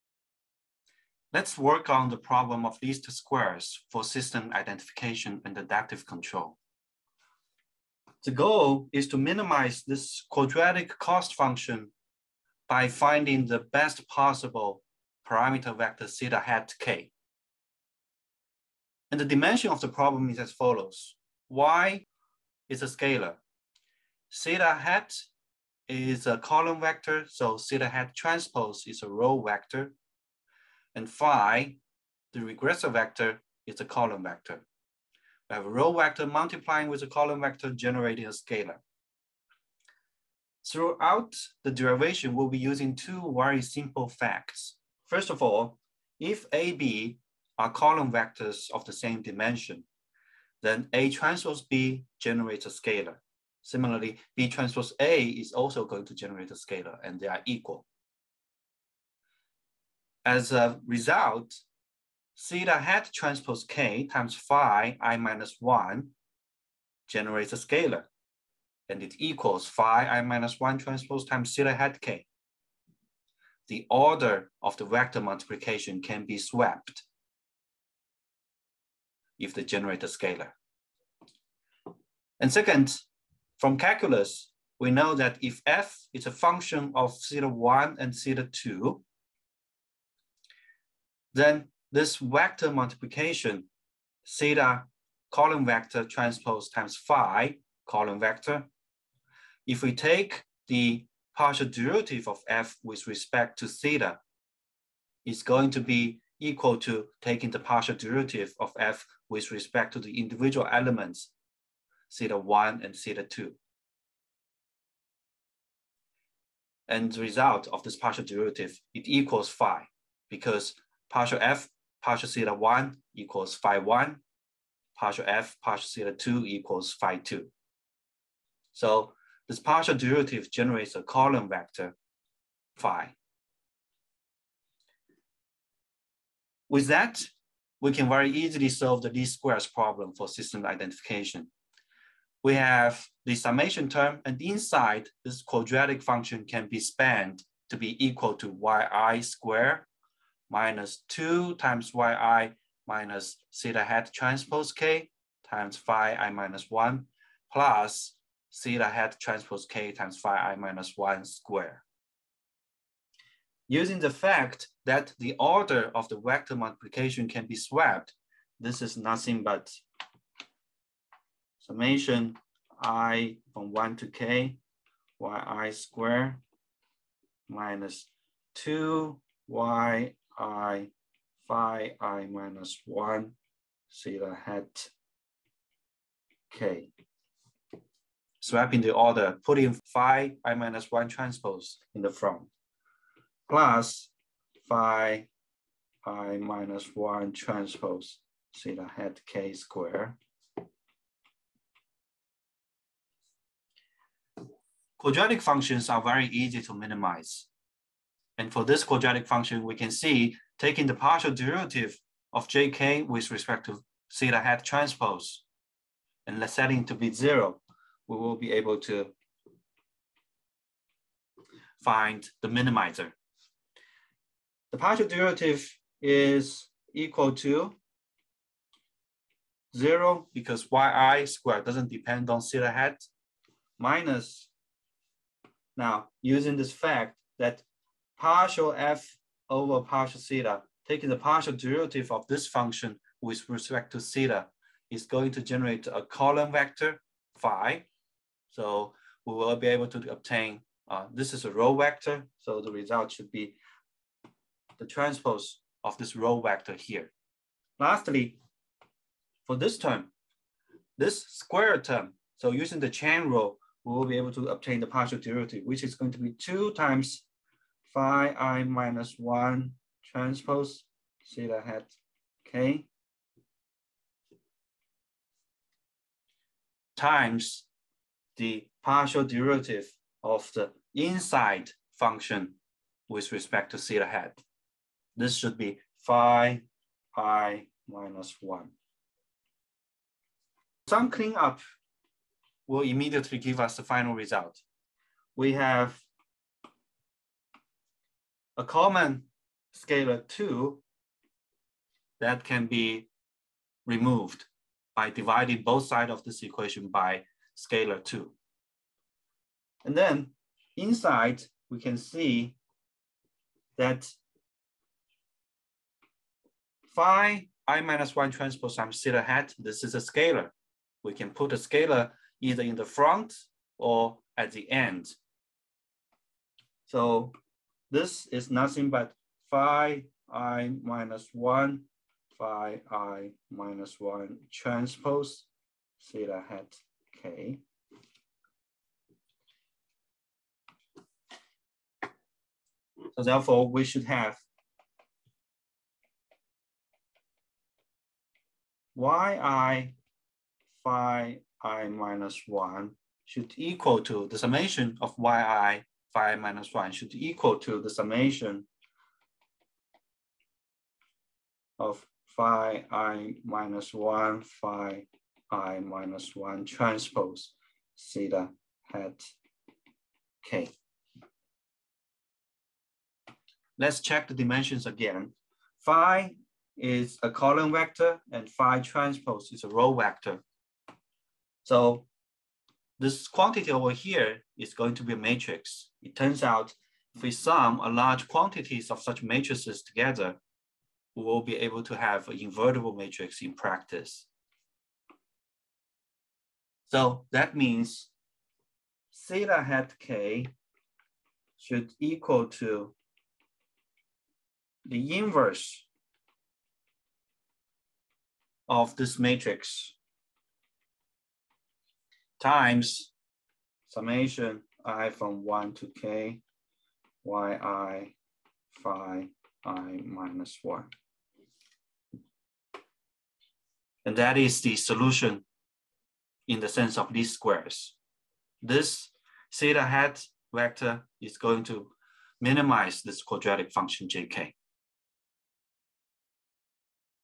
Least squares
UW ME 585 System Identification and Adaptive Control

⌨️ Topics: cost, quadratic functions, and optimization, in the context of least squares and system identification